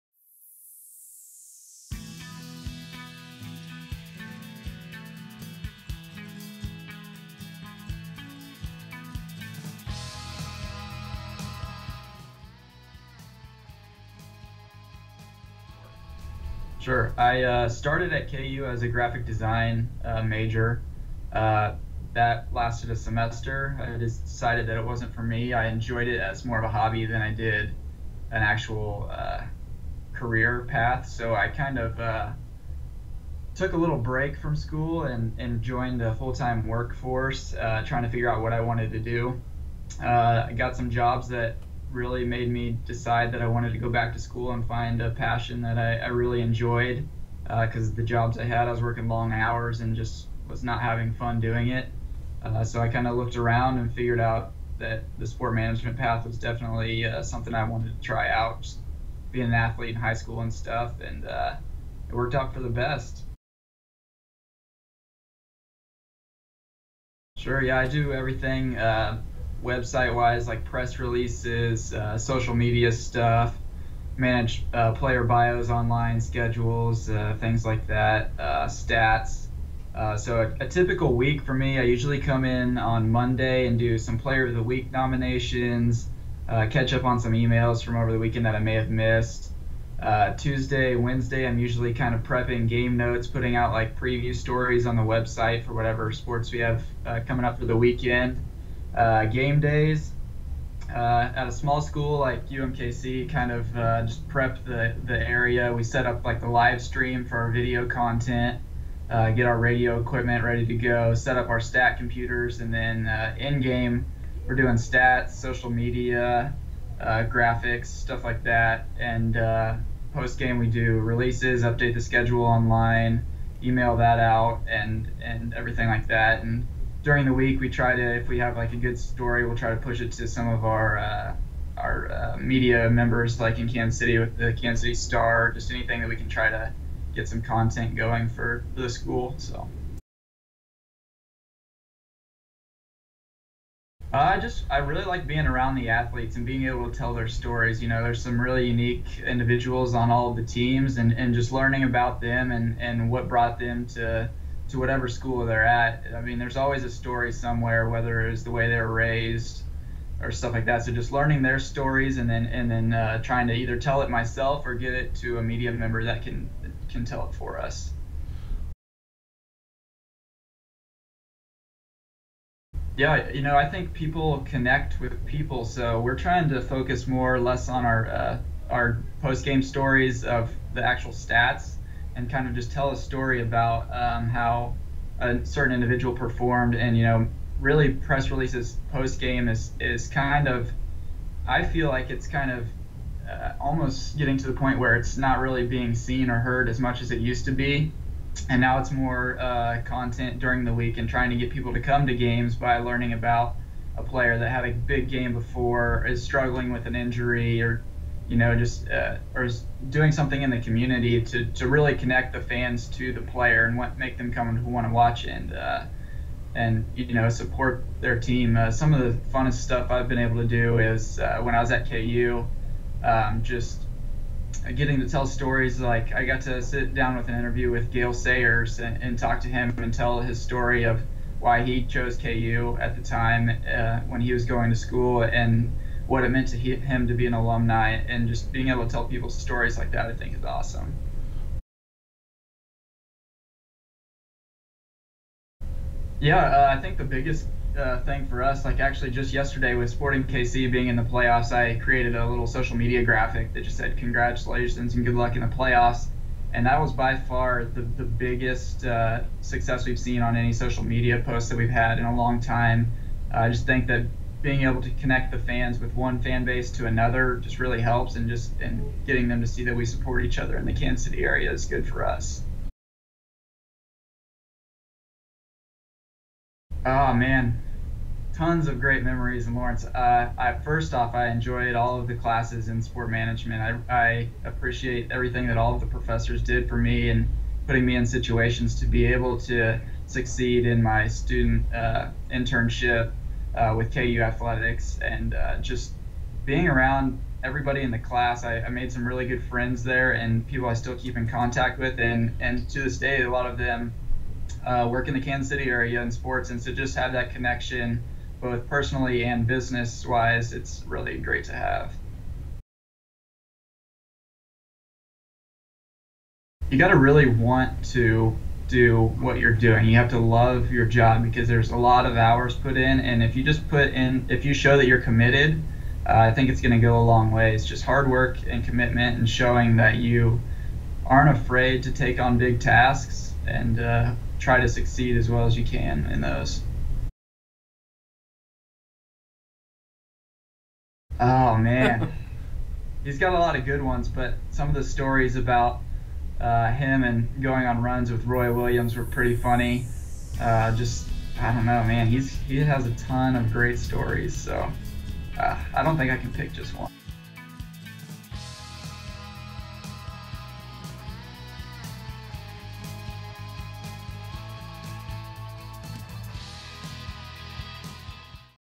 Alumni Spotlight - Josh Carson
Josh Carson is a 2015 KU Sport Management graduate who is currently the Assistant Director for Strategic Communications for the University of Missouri-Kansas City Athletics program